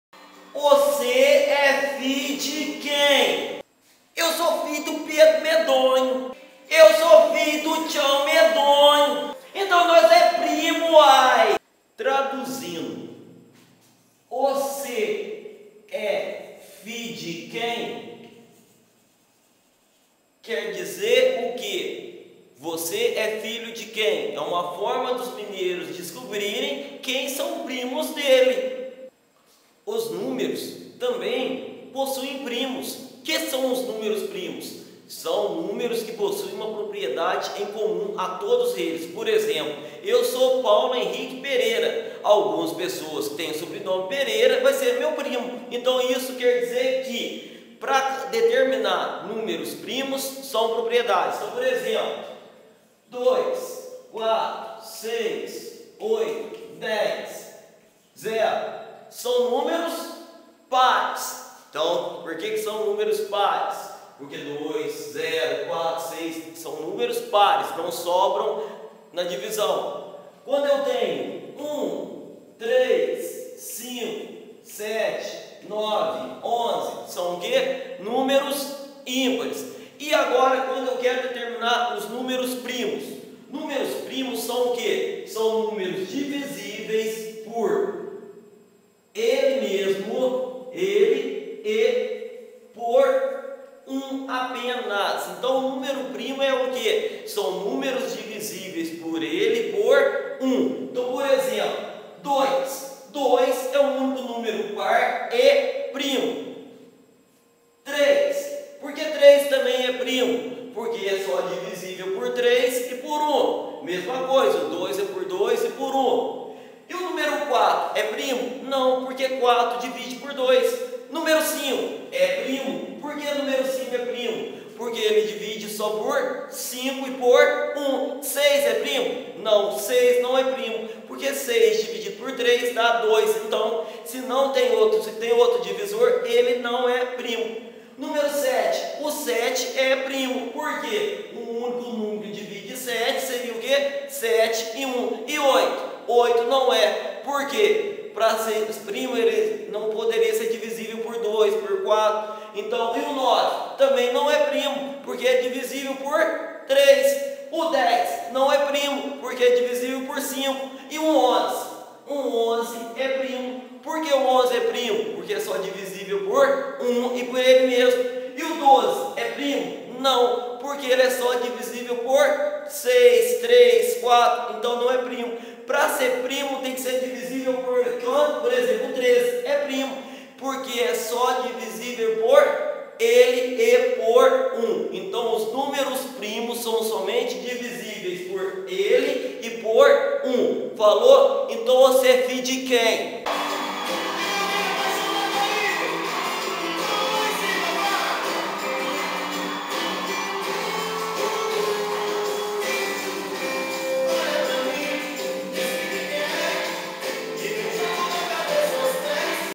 números primos com ph
Trabalhar com introdução de números primos com alunos do quinto e sexto ano do Ensino Fundamental.